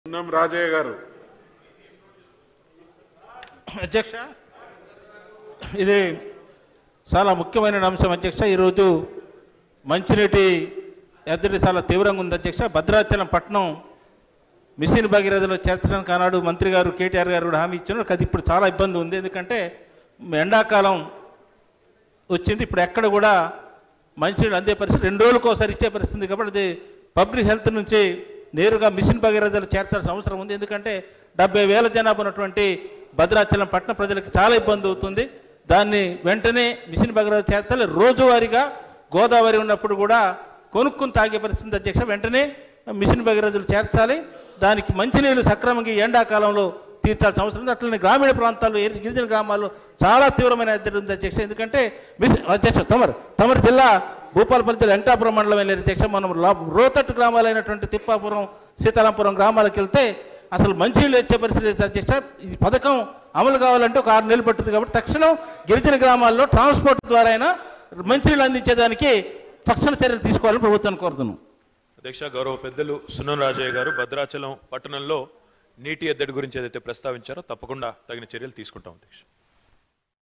CPM MLA Sunnam Rajaiah Demands Govt To Include Bhadrachalam In Mission Bhagiratha | TS Assembly | V6
CPM MLA Sunnam Rajaiah Demands Govt To Include Bhadrachalam In Mission Bhagiratha | TS Assembly.